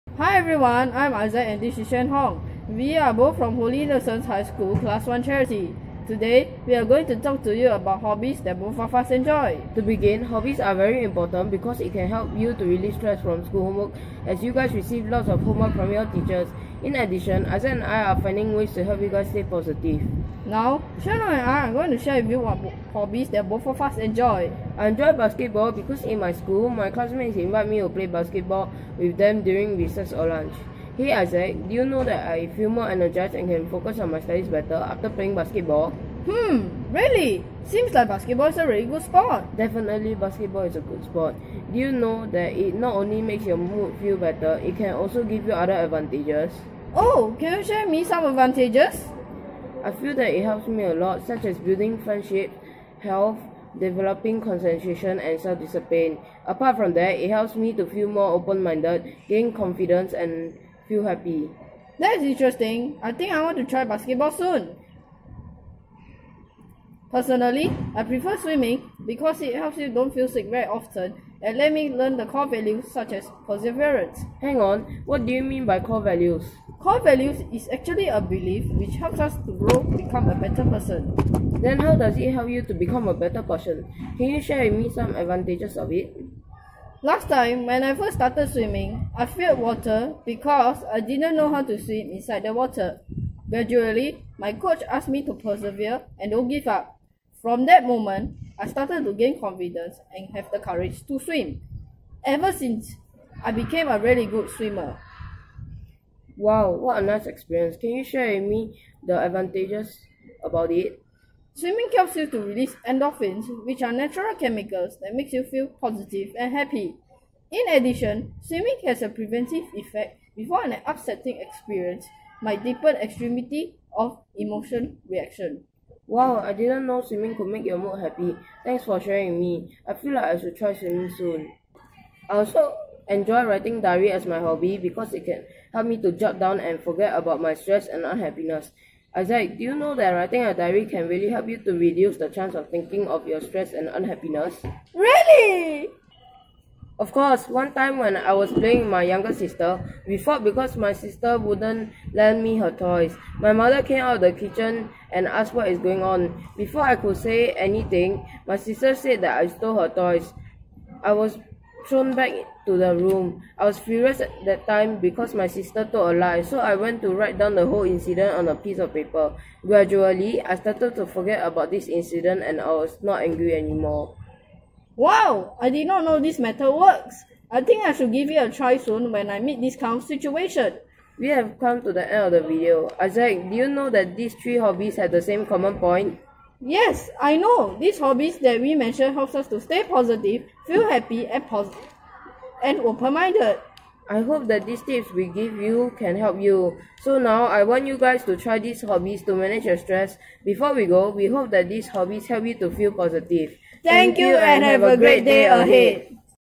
What hobbies do teenagers enjoy?(Done by Isaac Lau and Tay Xuan Hong) Holy Innocents' High School
This is a video done by two students from Holy Innocents'High School ,they are Isaac Lau and Tay Xuan Hong respectively. This video is meant to give advices to teenagers on how to relieve stress,stay positive and happy.  Hope you will enjoy this video and these tips may help you!